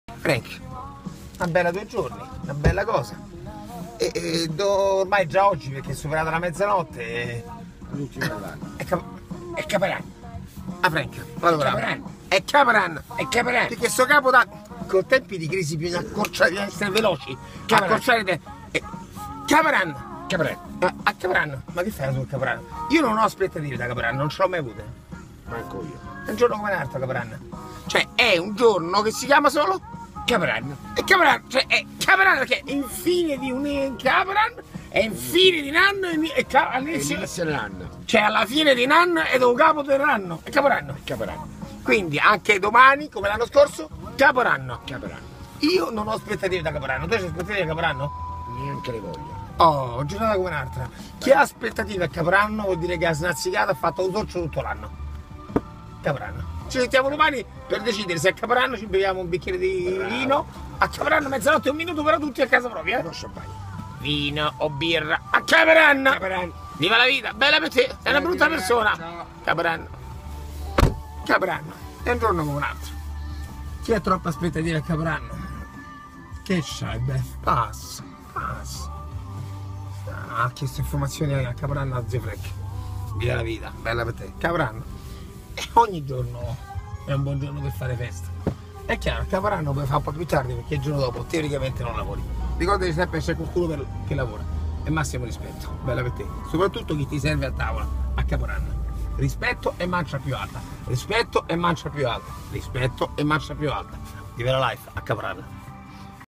Tutorial cAp(o)RaNN',Capodanno In Tempi Di Crisi. Stasera RISPETTO e Mancia Più Alta!
...secondotestomale...a cAp(o)RaNN' le Aspettative Salgono, Per Quale Arcano Motivo Non Si Sa, Er Criceto Tendente Al Sorcio Dovrebbe Capire Che È' Una Serata Come Un'Altra, Anzi Più Caotica e Pertanto Problematica...E Allora Viviamoci Sto cAp(o)RaNN' Senza Dargli Tutta Sta Responsabilità. Il 2015 Andrà Come lo Faremo Andare...secondotestomale
Vivalavita